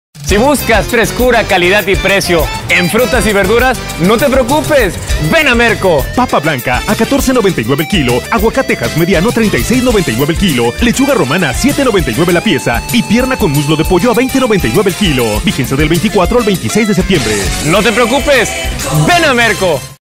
Preciazos en Frutas y Verduras. Vigencia: 24 al 26 de Septiembre. Zona Nuevo León
Ofertas válidas en Merco Zona Nuevo León.
Si buscas Calidad, Frescura y Precio ¡No te Preocupes! ¡Ven a Merco!
Vigencia: del 24 al 26 de Septiembre